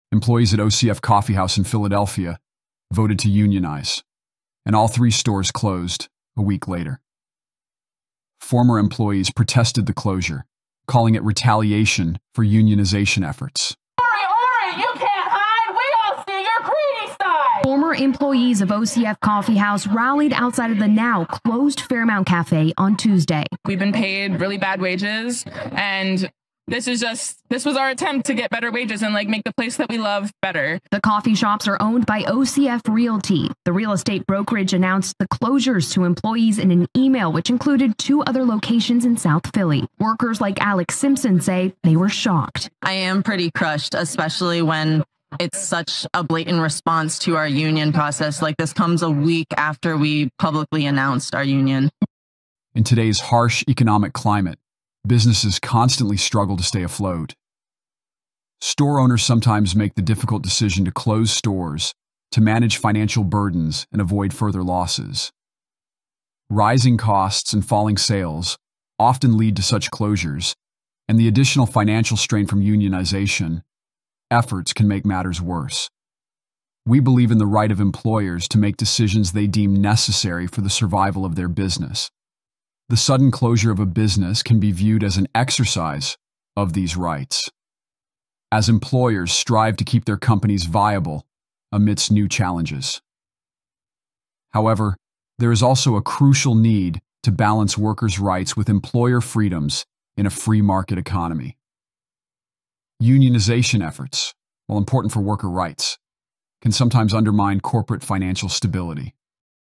Employees at OCF Coffee House Unionize, Stores Close Down
The shocking closure of OCF Coffee Houses after a union vote has left more than 30 employees out of work in Philadelphia. Employees at the OCF Coffee House in Philadelphia voted to unionize, aiming for better wages and benefits. However, just one week later, all three stores were abruptly closed down by OCF Realty, the owning company. The sudden shutdown has sparked protests and controversy, with former employees rallying outside the closed cafes, demanding answers and fair treatment. This video delves into the reasons behind the closures, the employees' reactions, and the ongoing protests. Watch to understand the full story of this unfolding labor dispute and the impact on the local community.